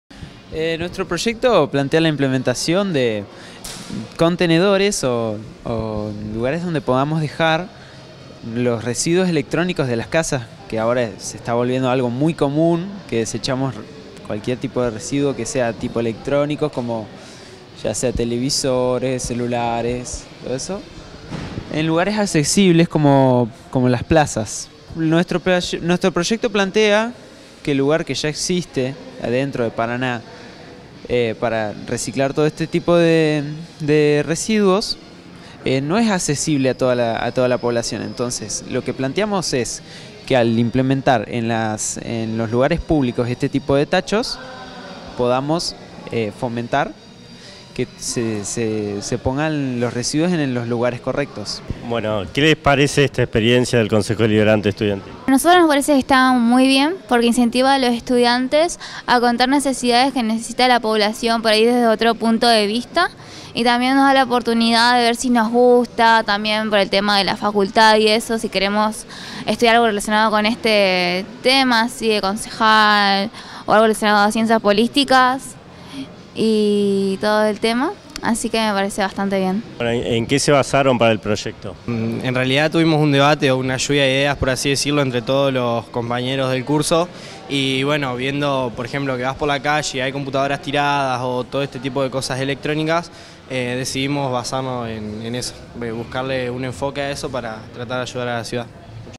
Proponen ubicar contenedores de residuos electrónicos en plazas
En el marco de la Sesión Preparatoria del Concejo Deliberante Estudiantil de Paraná, representantes del Colegio Paraná High School comentaron su ante-proyecto de colocación de residuos electrónicos en plazas y paseos públicos de la ciudad. Además valoraron como positiva la posibilidad de participar y elaborar propuestas para mejorar la ciudad.